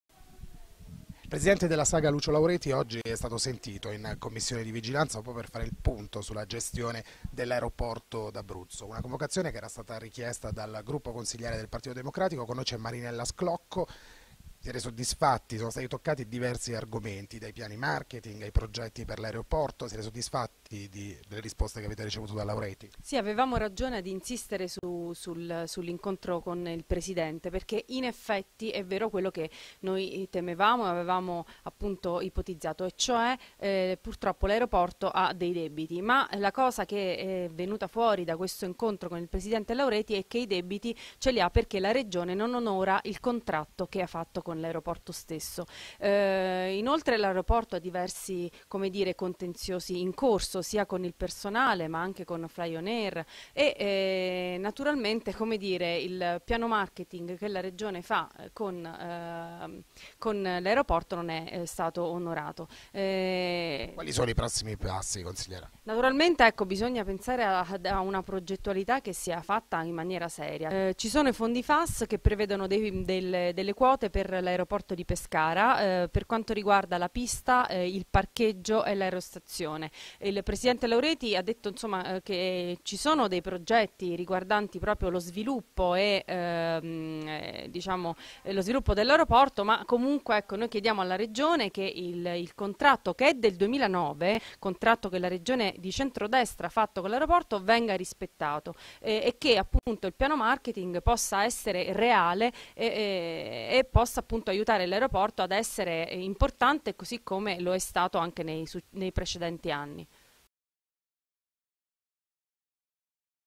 Sclocco su audizione in Vigilanza del Presidente Saga
Il Presidente della Saga Lucio Laureti è stato sentito in audizione in Commissione di Vigilanza per fare il punto sulla situazione dell'Aeroporto d'Abruzzo. Una convocazione richiesta del gruppo consiliare del PD. Ai nostri microfoni la Consigliera Marinella Sclocco.